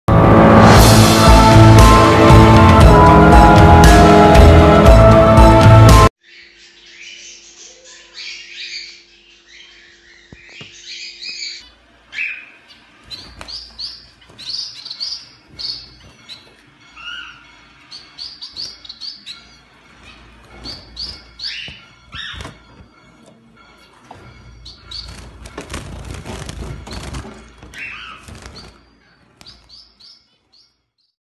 Birds are hand tamed and open place flying practice on top of the home
Its simply bird video but thumbnail is make edition. So viewers are dont angry with me pls.

Pls more support me.

Thanks.